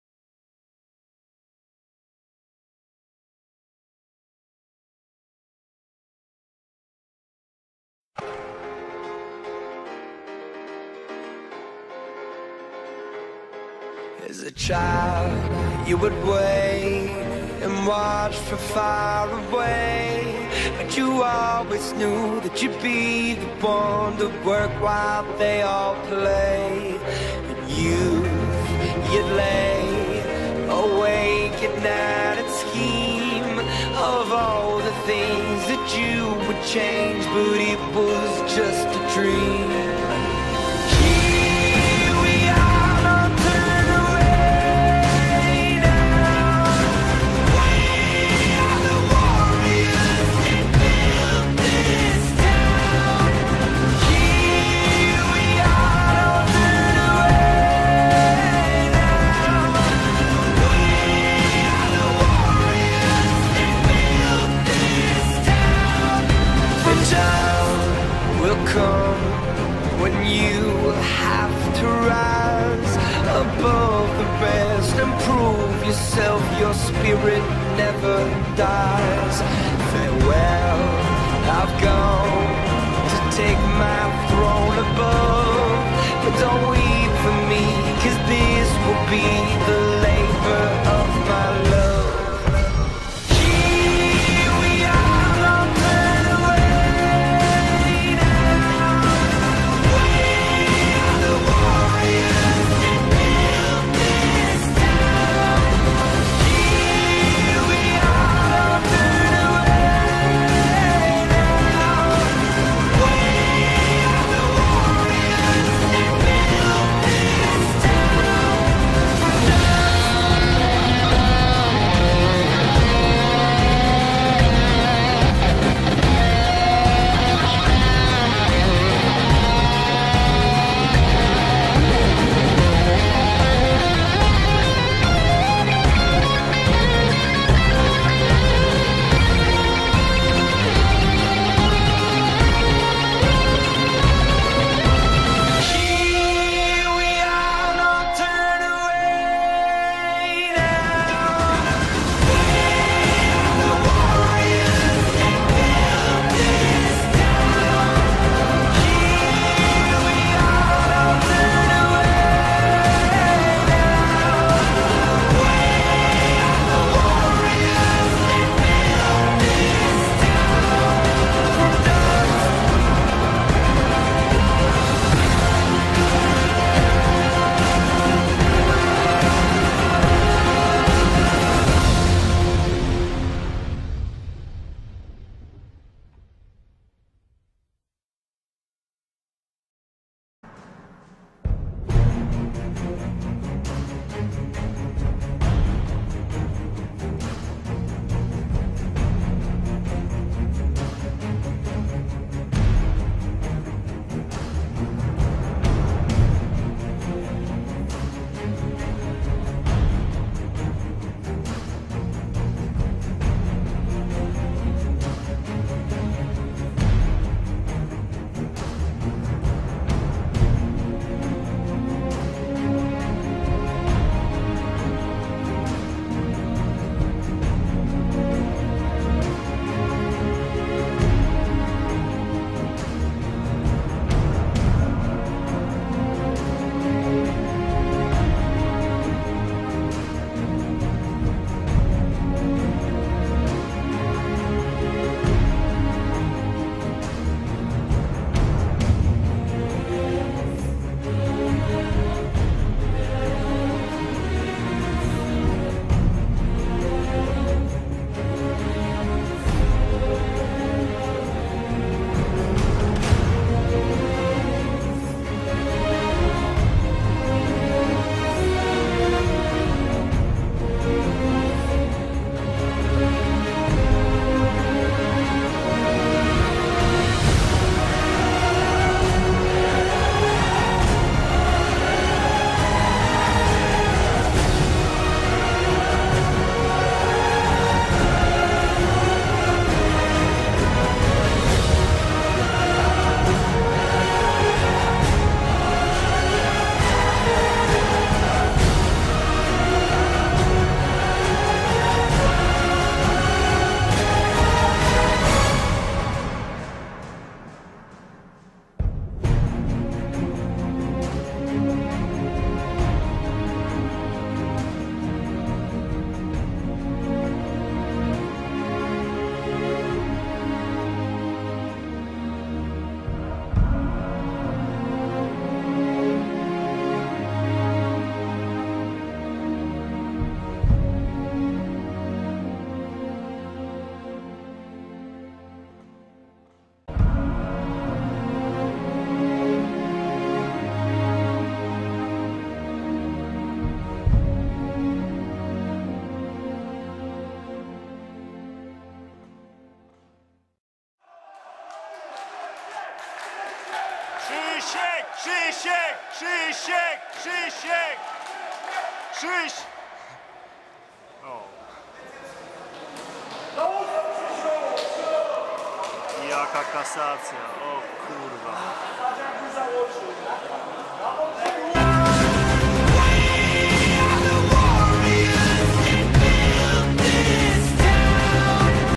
Gala Boksu Łącko 06.06.2015 Walka Wieczoru
Krzysztof Twardowski vs Bartosz Zuzela